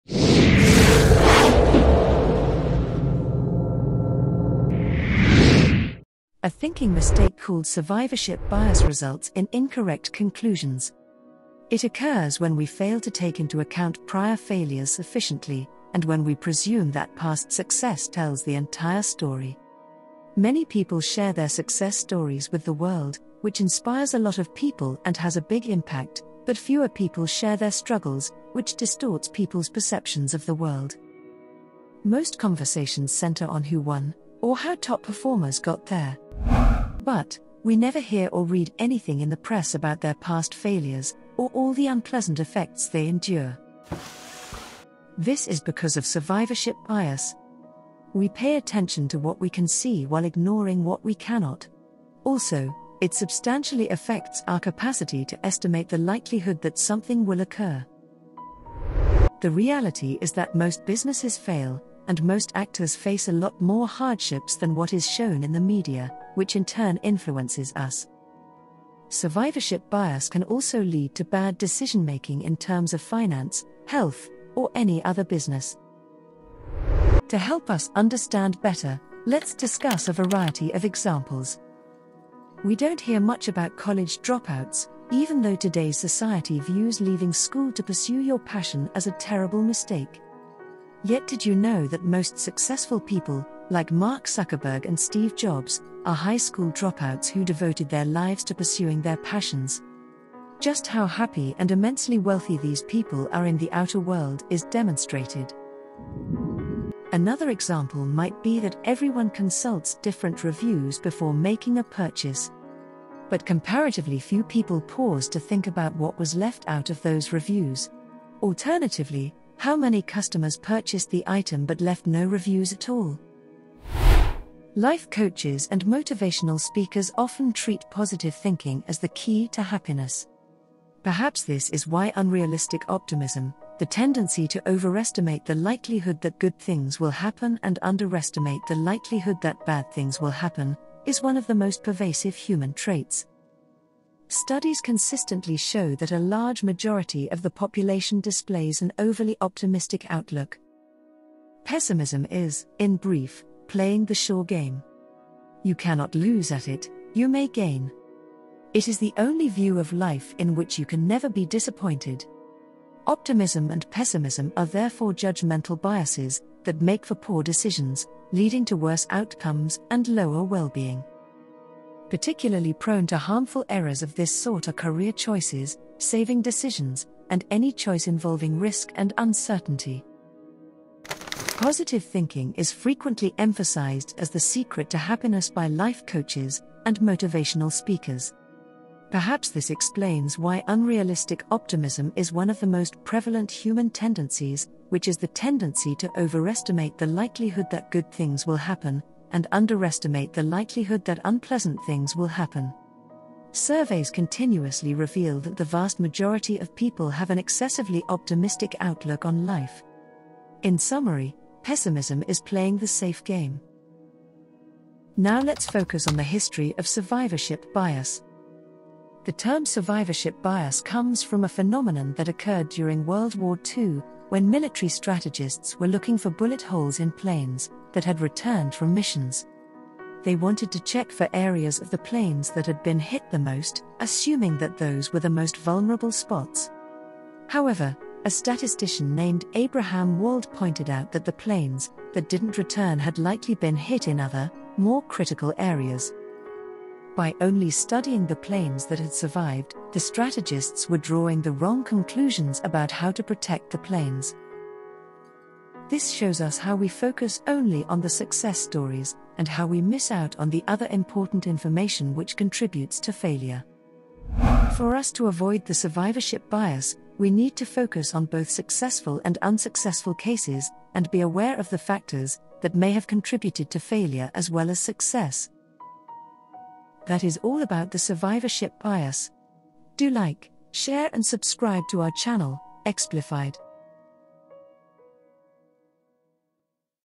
The Survivorship Bias: Why Success Stories Don't Tell the Whole Story
Survivorship bias, also known as survivor bias, is the tendency to overestimate successes and undervalue failures. Most of us tend to follow the positive side of story, which is the success story rather than the negative one. In turn failing us to know about other important information.